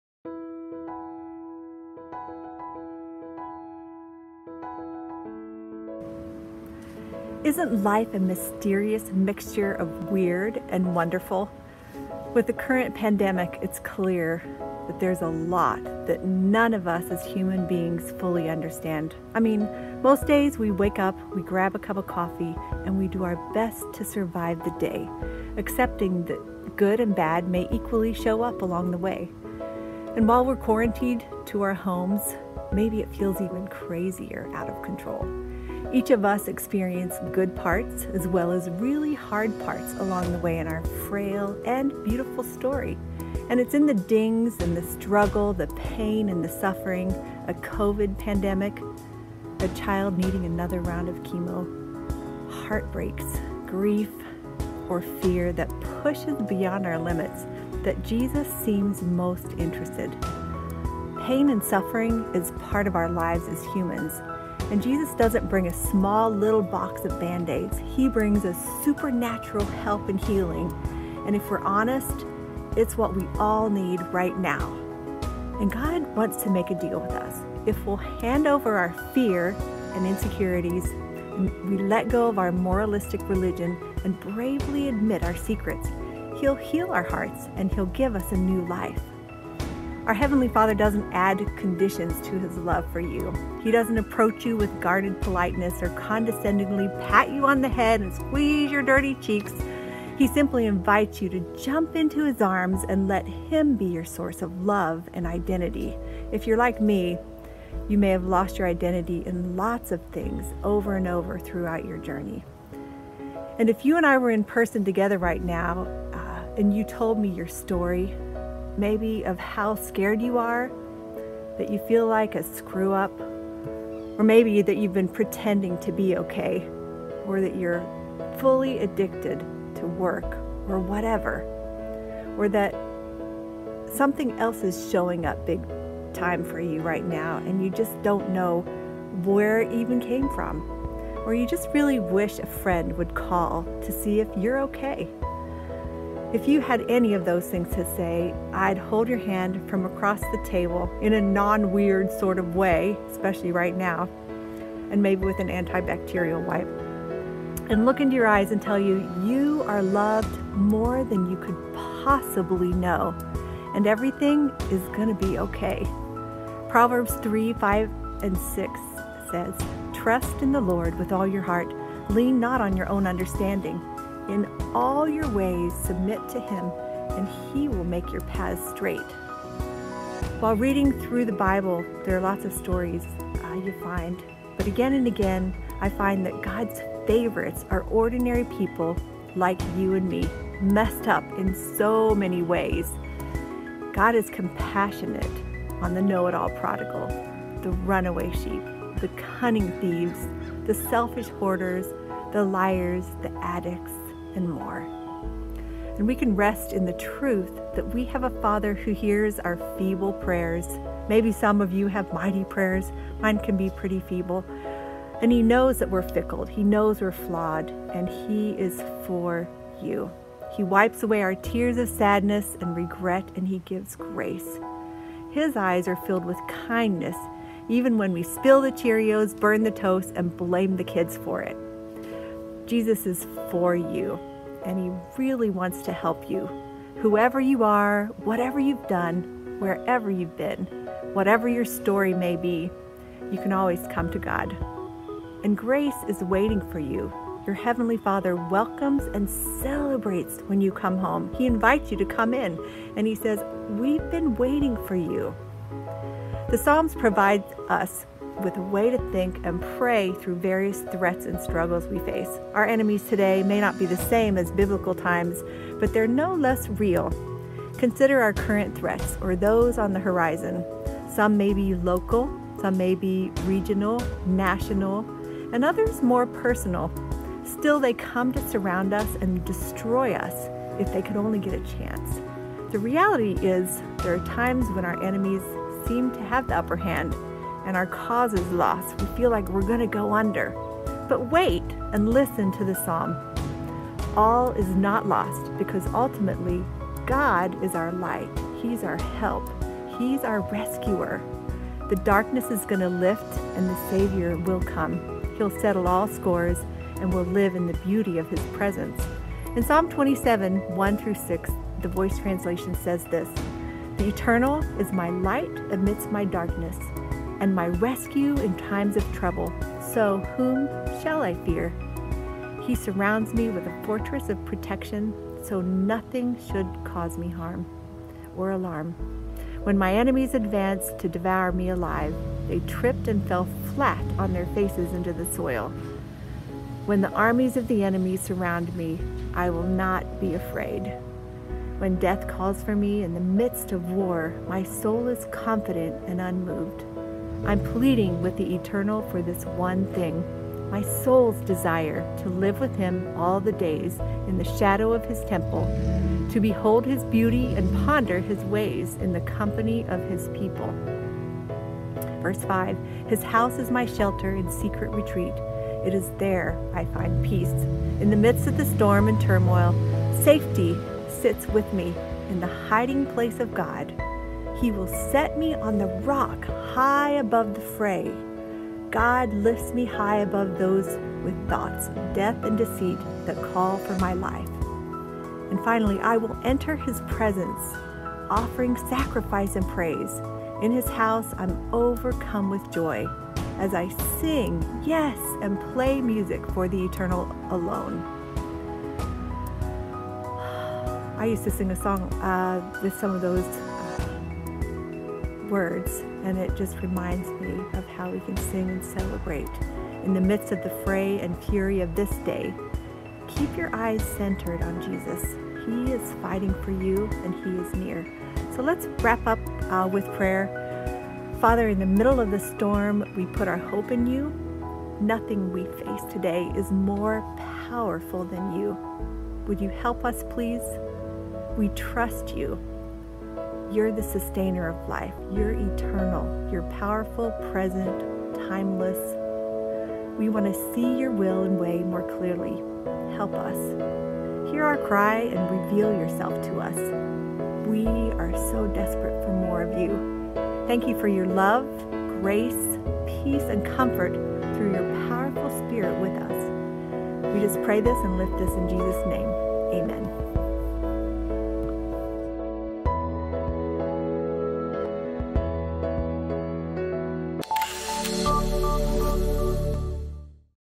Online Devotional with Barb
Start your day with our Connections Pastor, Barbara Wilkinson as she  talks about ways to overcome real-life struggles.

ONLINE LIVESTREAMING SERVICE TIMES
Sundays @  9:15am and 11:15am